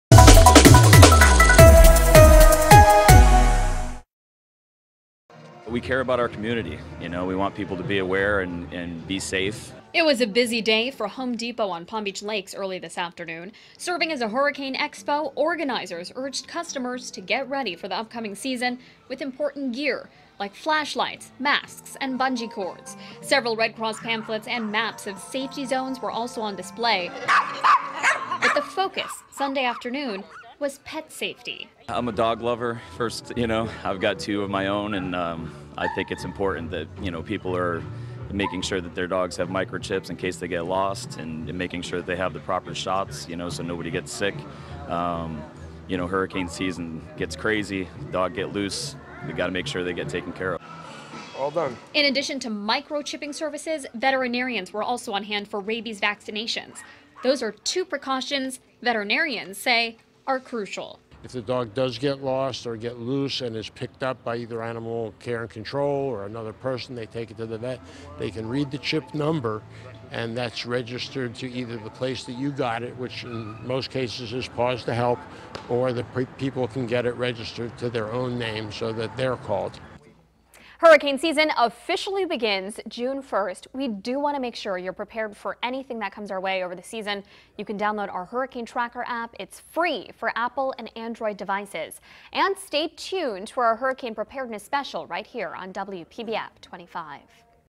Preparing your pet for hurricane season
A look at how to prepare your pets for hurricane season. Sanika Dange reports.